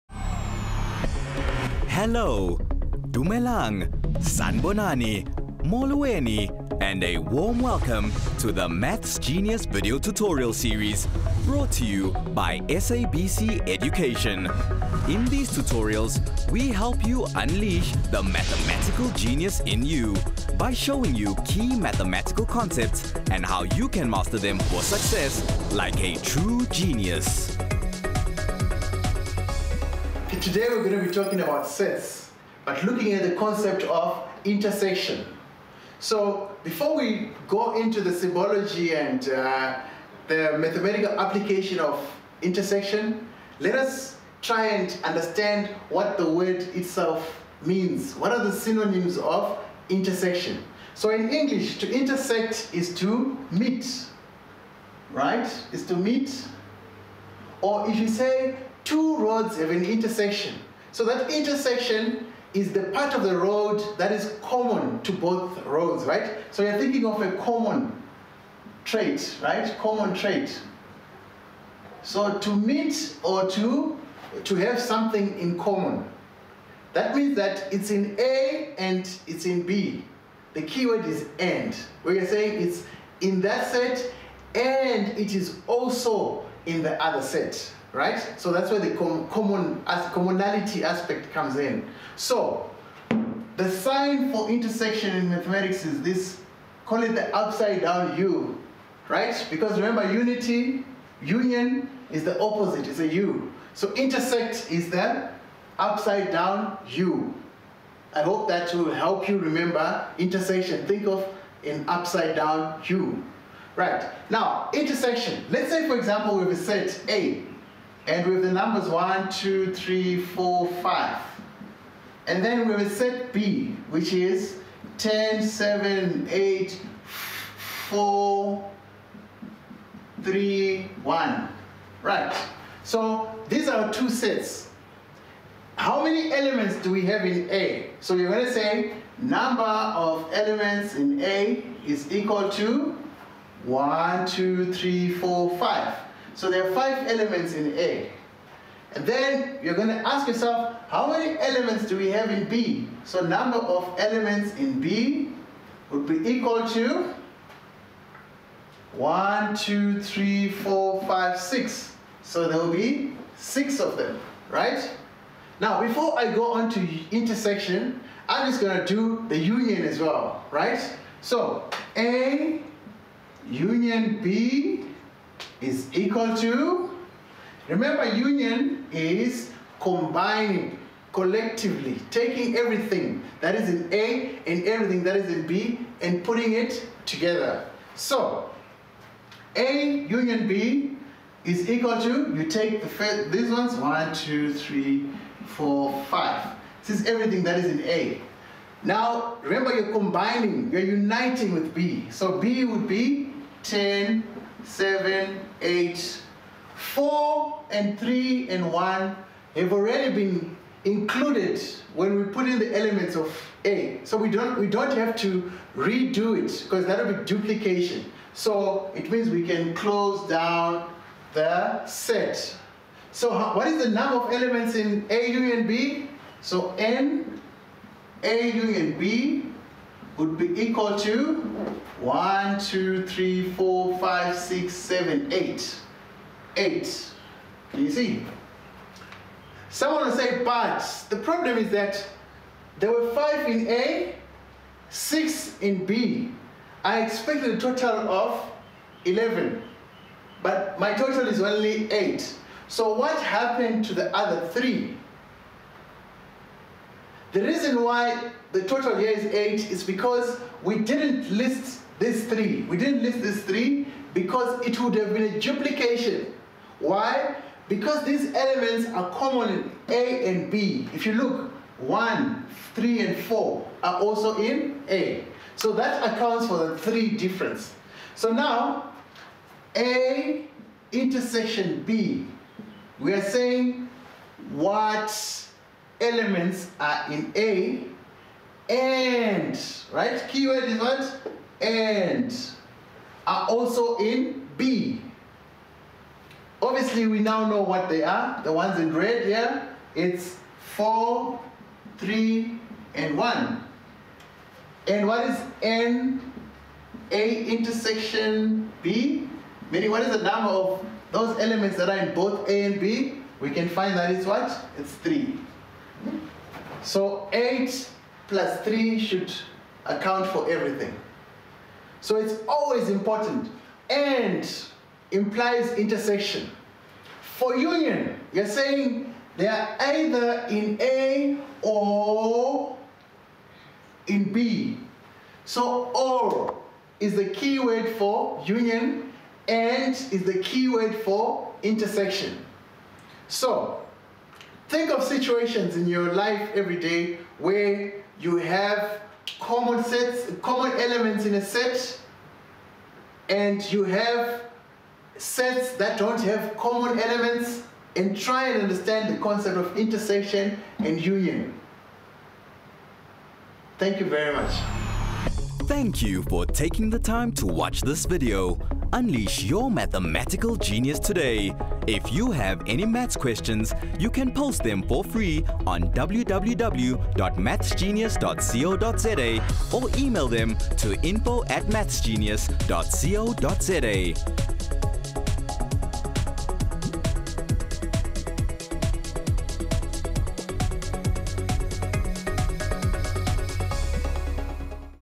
Maths Genius: Episode 20 - Sets: Intersection
Sets: Intersection
Sets are very pivotal in mathematics and this lesson focuses on the idea of intersection which is the set of elements common to the sets under consideration